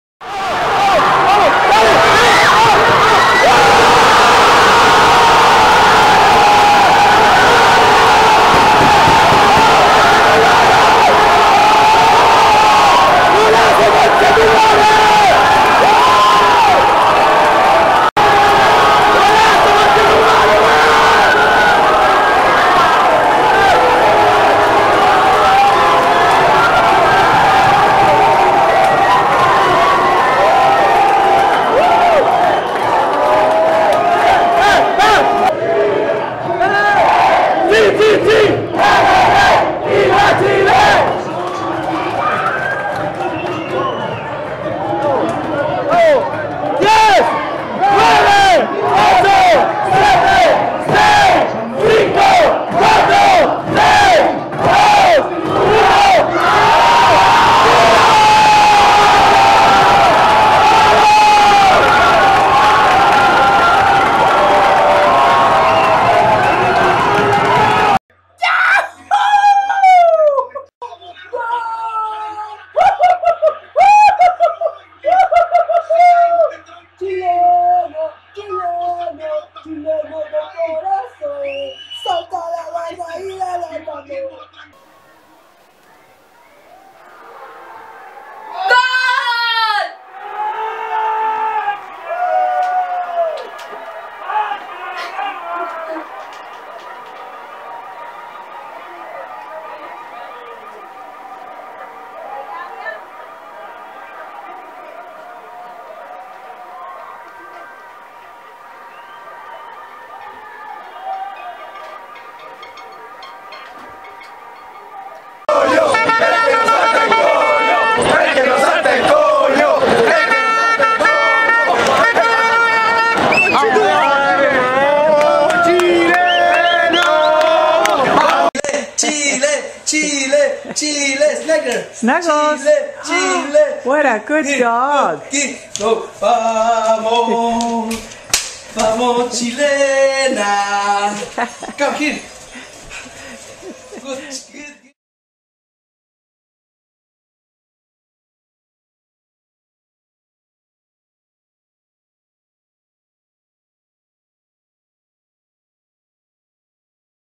Chilean Fans go crazy after amazing 2:0 win over Spain! - Chile vs Spain - FIFA World Cup 2014
Chilean Fans go crazy after amazing 2:0 win over Spain! (Fan Reactions)

Chile vs Spain, June 18, FIFA World Cup Brazil 2014